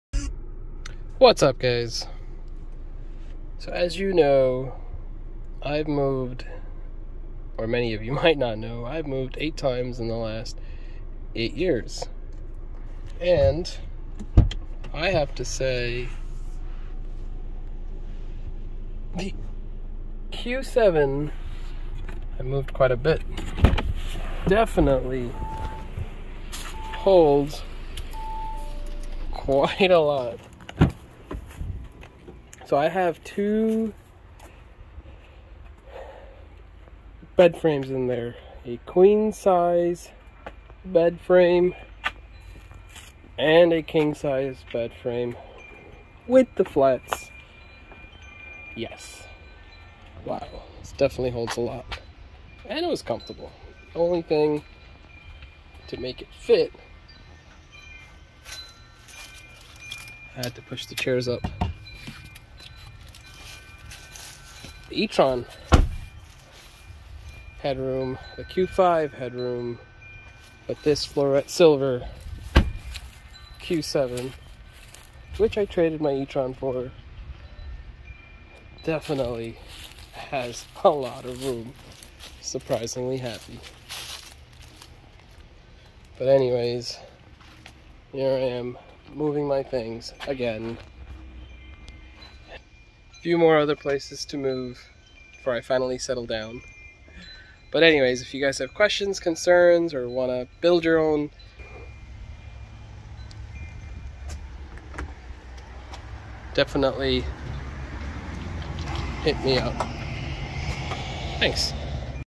The 2022 Audi Q7 Florett Silver has plenty of room to move things around in.
needed to move a king size and queen size bed frame, plenty of room to move things around with the Q7.